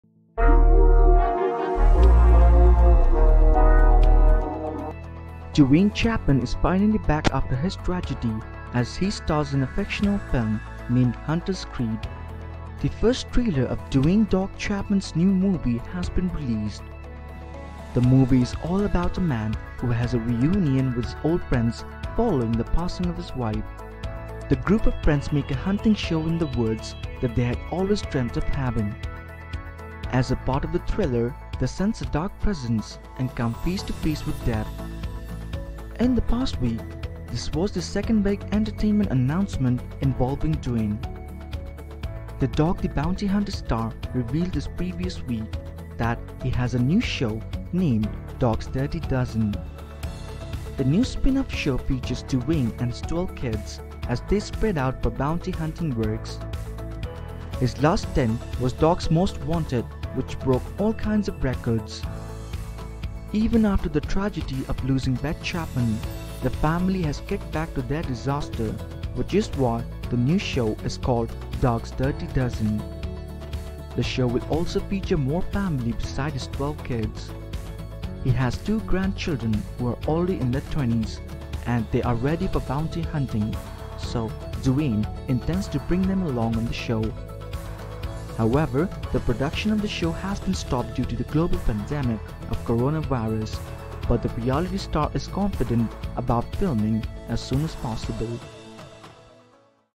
Duane Chapman is back with new movie Hunter's Creed
Duane Chapman made a big announcement regarding his two big entertainment involvement after his tragedy. The trailer of his movie named Hunter's Creed has been released while he has a new show named DO's Dirty Dozen. Though the productions have been halted, he is confident about filming very soon.